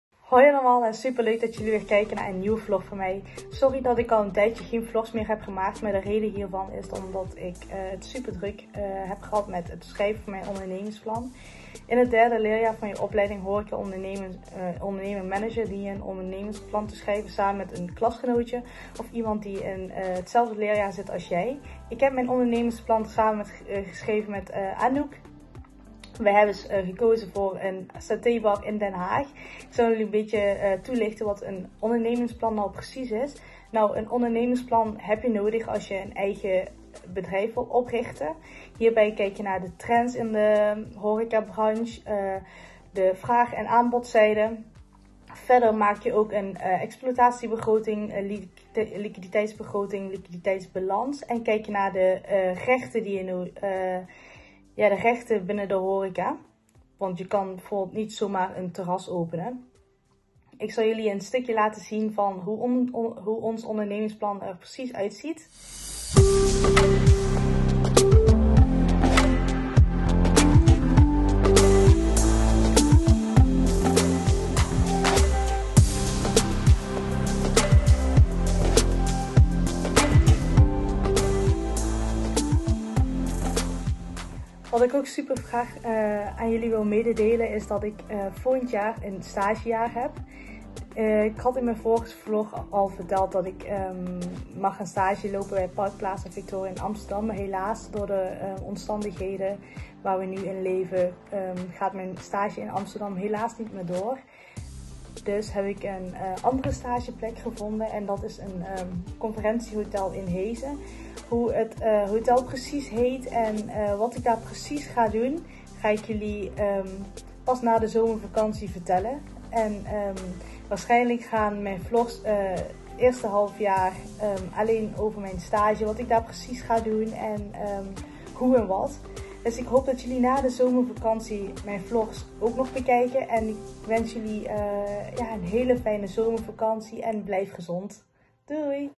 Amm is druk bezig geweest met haar ondernemingsplan
Vlogger Amm van Mbo Horeca van De Rooi Pannen Eindhoven is druk bezig geweest met haar ondernemingsplan en heeft nieuws over haar stage.